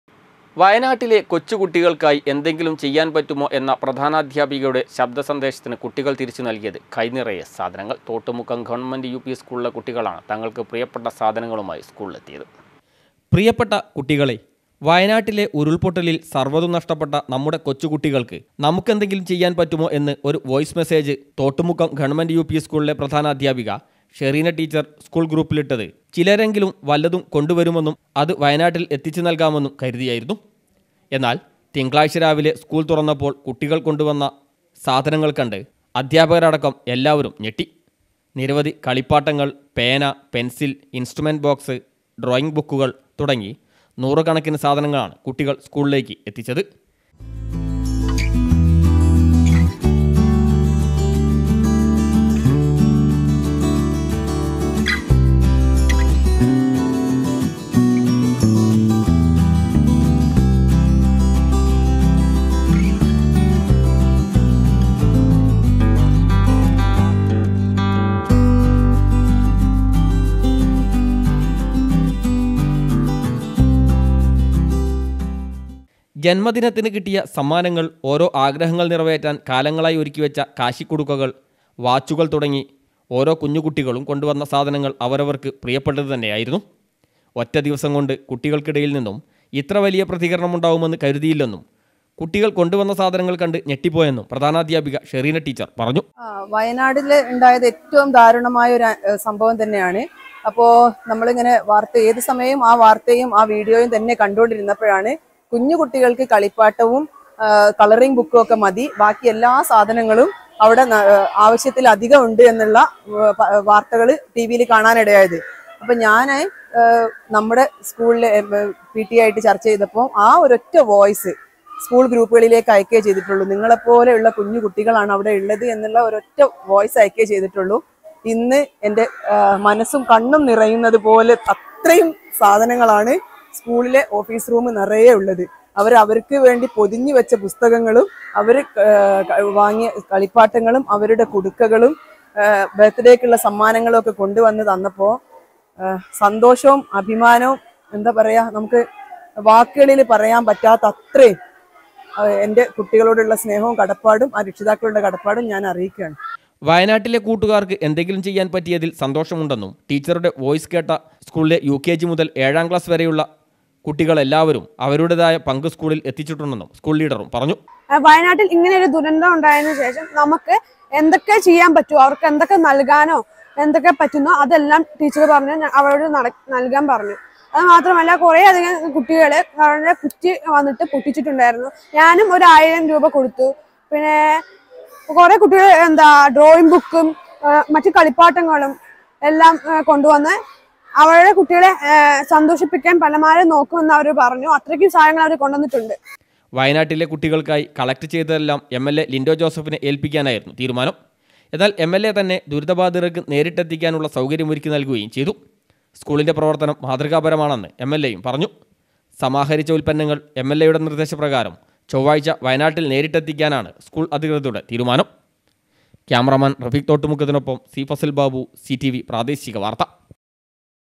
വയനാട്ടിലെ കൊച്ചു കുട്ടികൾക്ക് എന്തെങ്കിലുംചെയ്യാൻ പറ്റുമോ എന്ന പ്രധാനാധ്യാപികയുടെ ശബ്ദ സന്ദേശത്തിന്
വയനാട്ടിലെ കൊച്ചു കുട്ടികൾക്ക് എന്തെങ്കിലും ചെയ്യാൻ പറ്റുമോ എന്ന പ്രധാനാധ്യാപികയുടെ ശബ്ദ സന്ദേശത്തിന് കുട്ടികൾ തിരിച്ചുനൽകിയത് കൈനിറയെ സാധനങ്ങൾ.
തോട്ടുമുക്കം ഗവർമെന്റ് യു പി സ്കൂളിലെ കുട്ടികളാണ് തങ്ങൾക്ക് പ്രിയപ്പെട്ട സാധനങ്ങളുമായി സ്കൂളിലെത്തിയത്